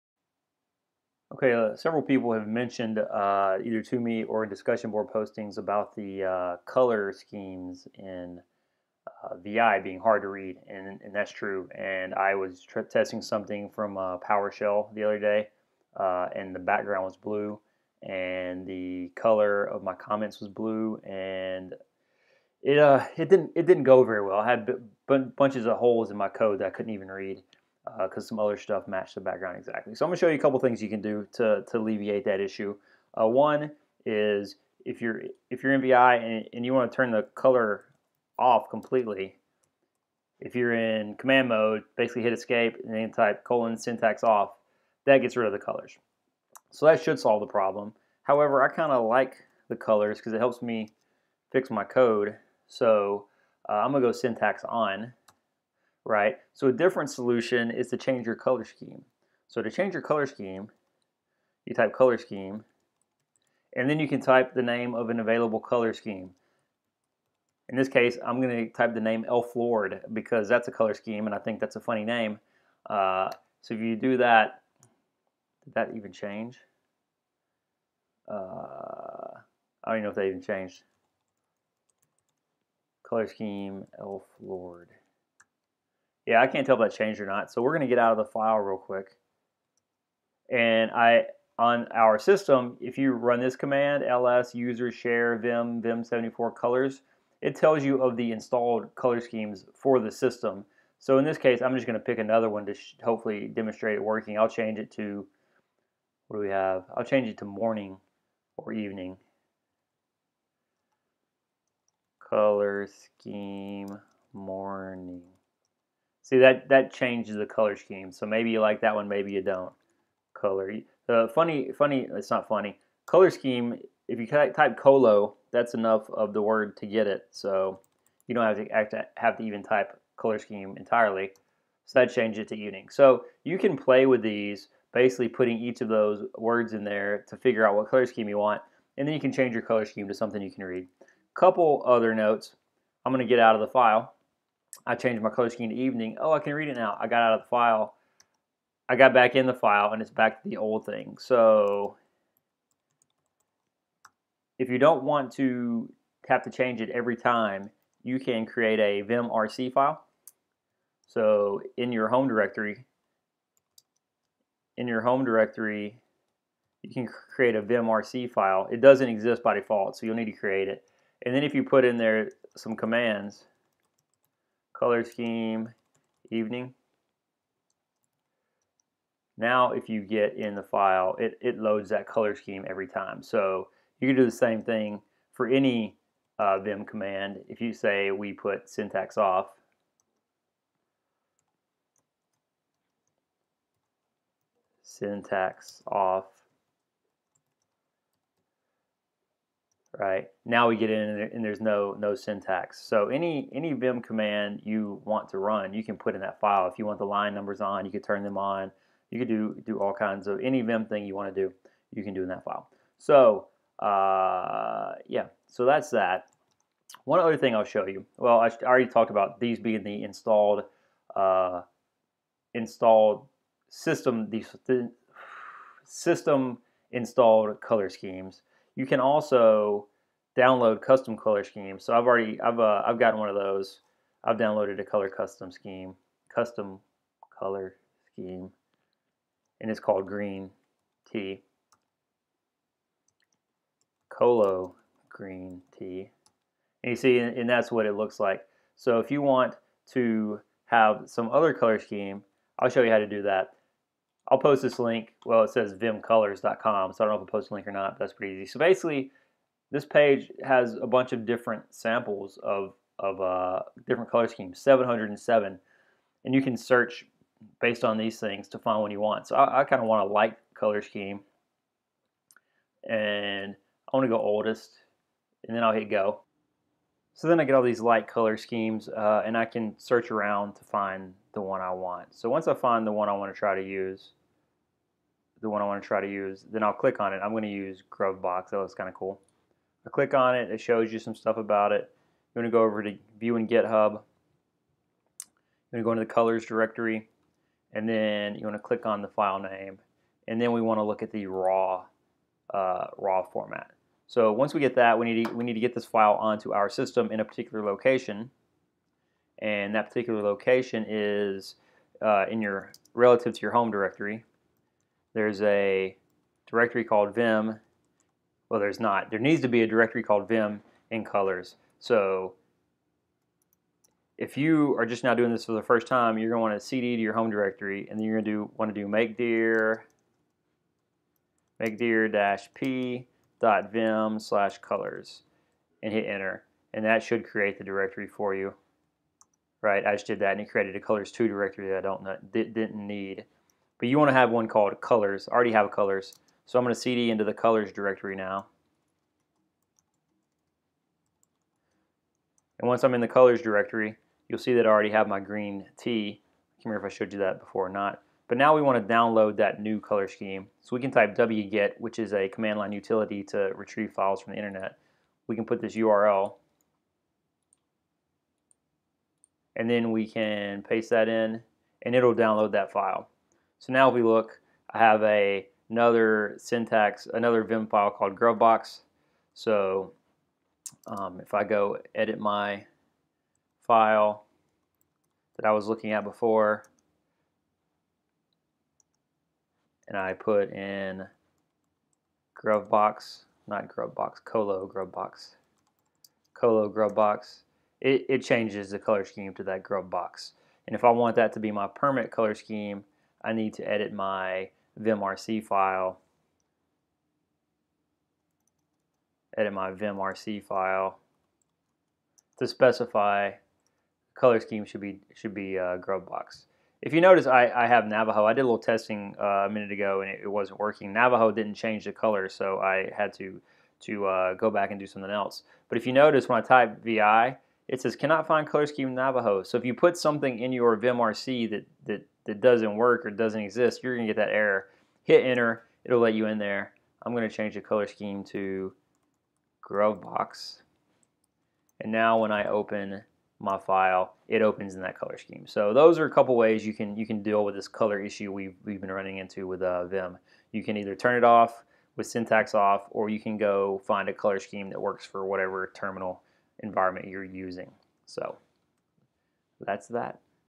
vim colors
How to turn off the syntax coloring, and/or change your colorscheme

The site I use to find additional colorschemes in the video is no longer there... I mean there is something at the domain name, but it's not what it used to be.  Here is one that you can try instead